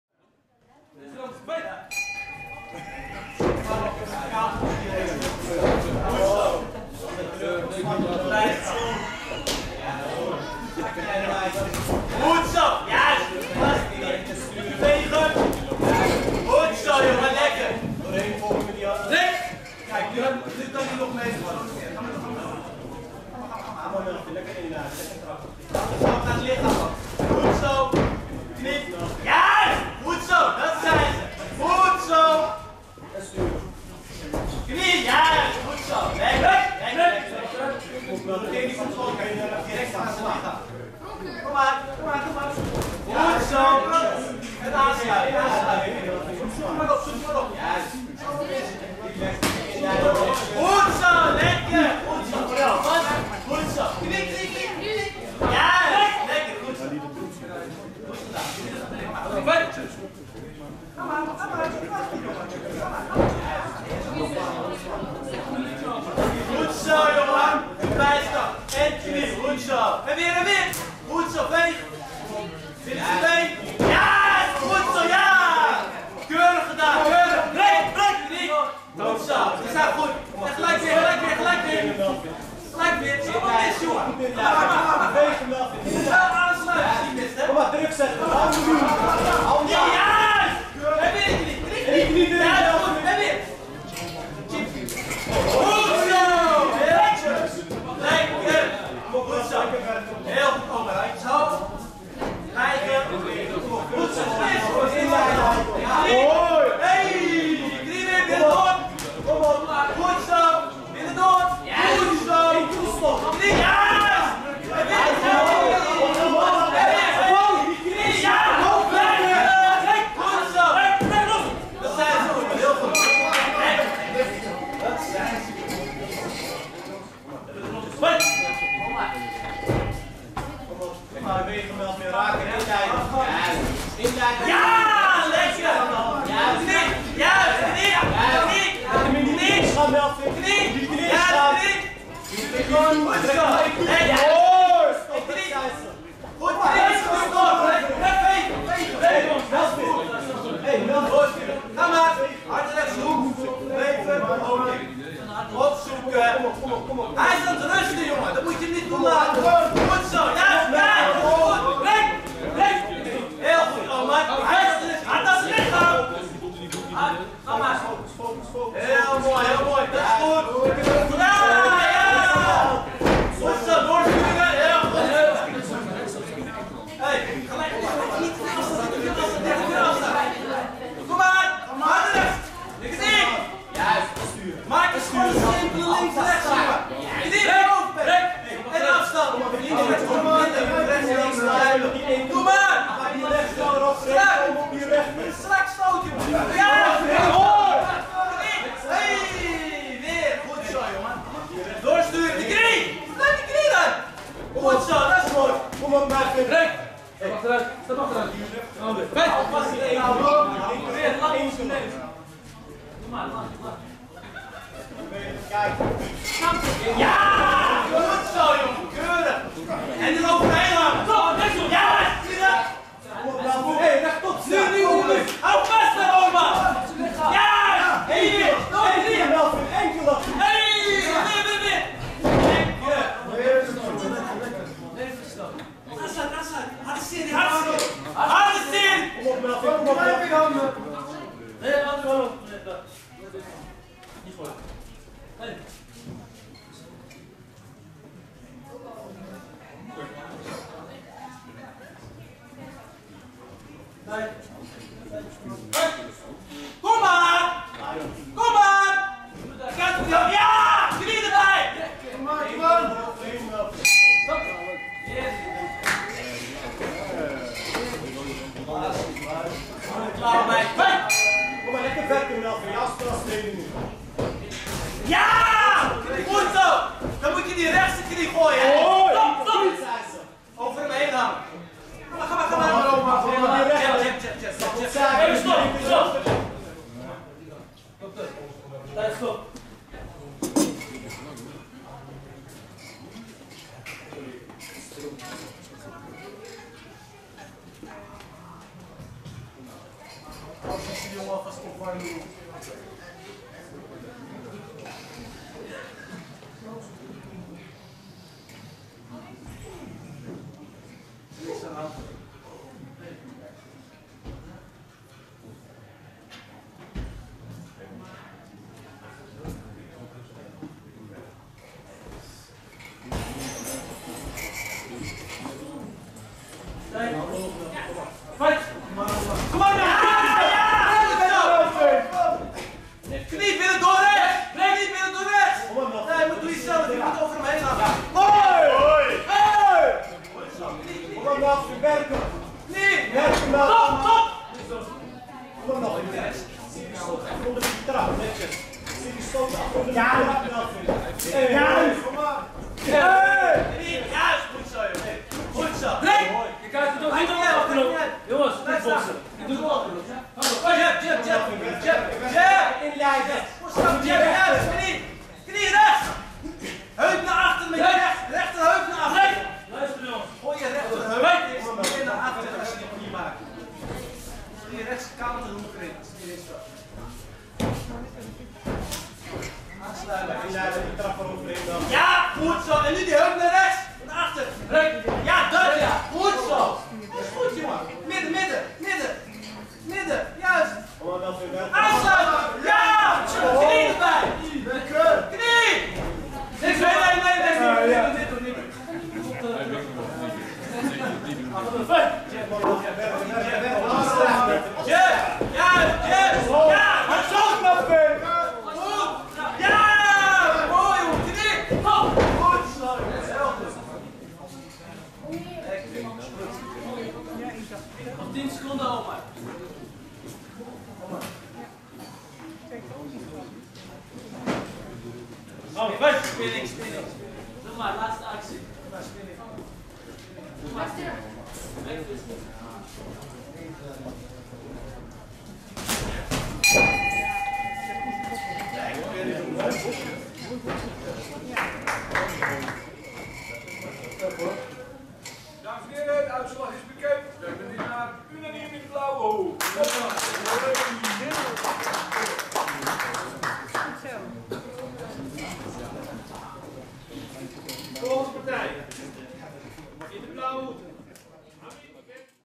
Melvin Bonsu vs Omar Mahgoub
Mini competitie C klasse
Deelnemers:
Bonsu
el Kaderi
Mahgoub
Challenger Toernooi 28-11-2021 Den Haag